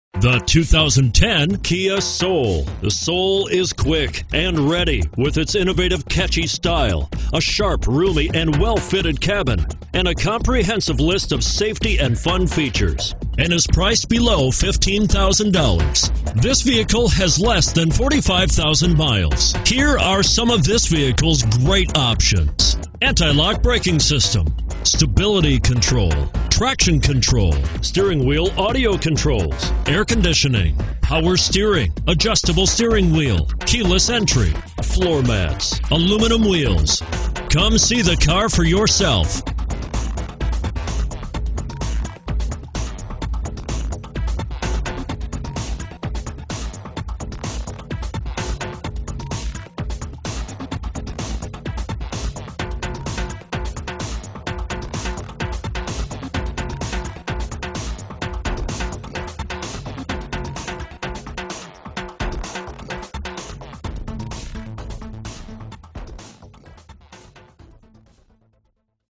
2010 KIA SOUL Cherry Hill, NJ 76103
2010 KIA SOUL Cherry Hill, NJ
Stock# 76103

Looking for the right car? Today could be your lucky day. With 43144 miles, this Java, 2010 Kia Soul equipped with Automatic transmission could be yours. Request more information and set up a test drive right away.